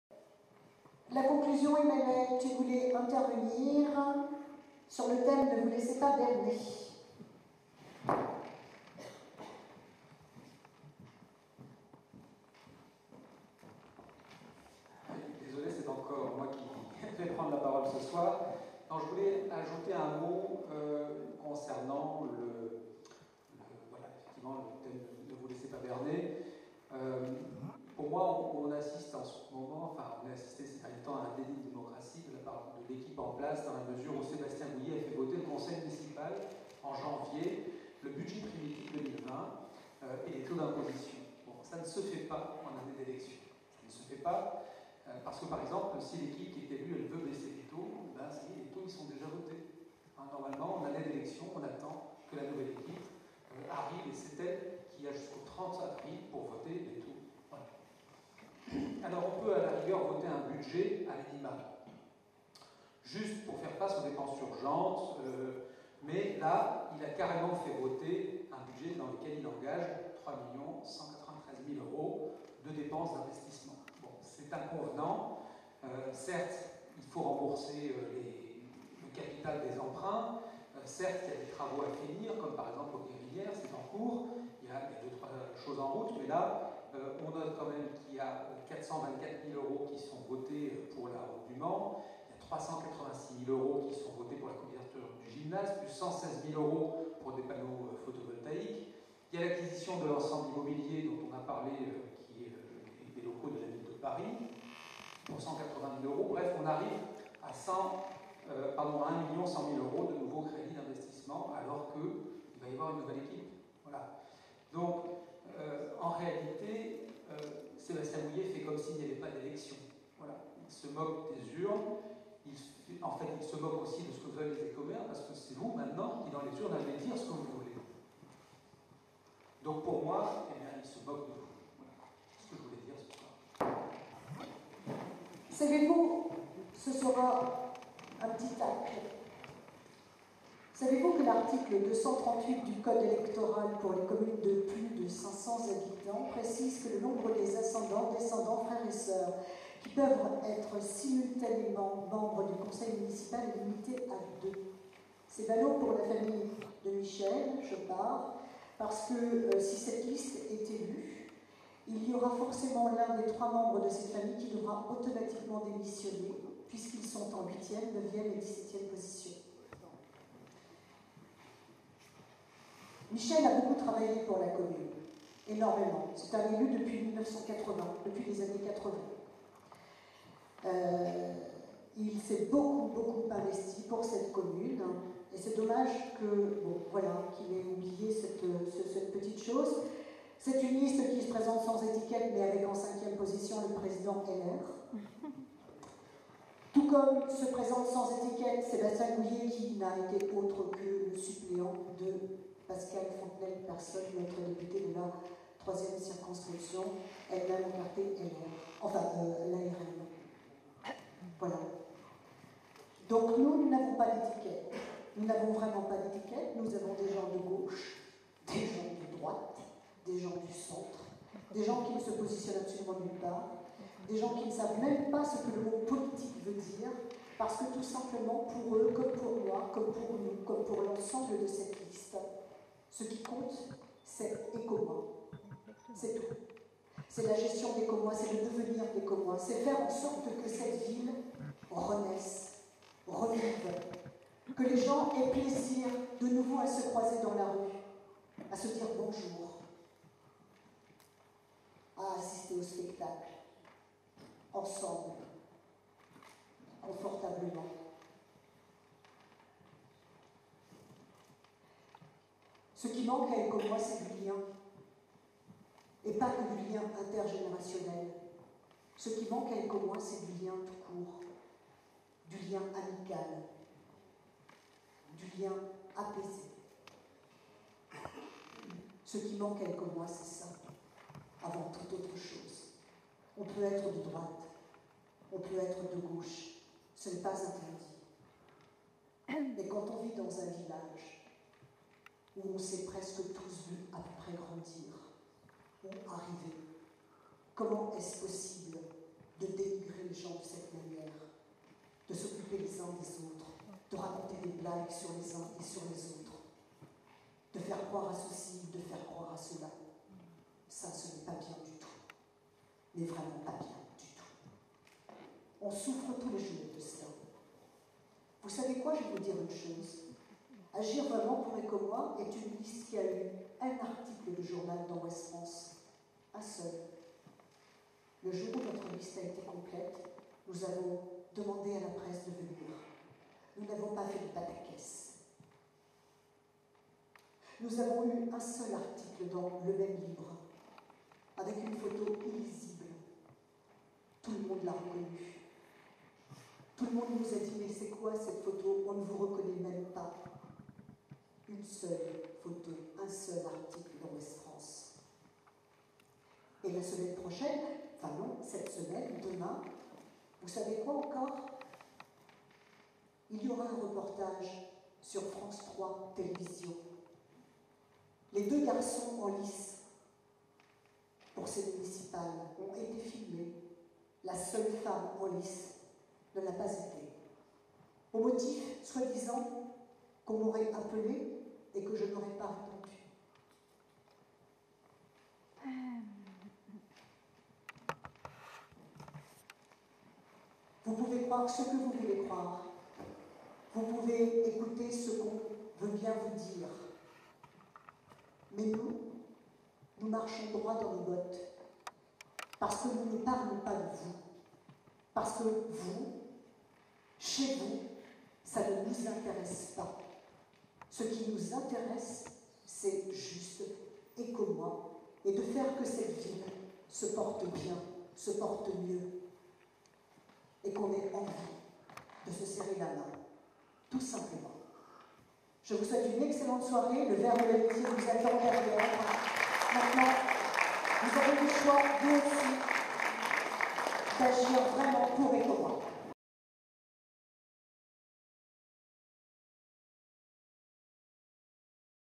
10 Conclusion ce qu'il manque à Ecommoy
Extrait de la réunion publique AGIR VRAIMENT POUR ECOMMOY du 9 mars 2020, salle polyvalente à Ecommoy. Conclusion sur ce qu'il manque à Ecommoy.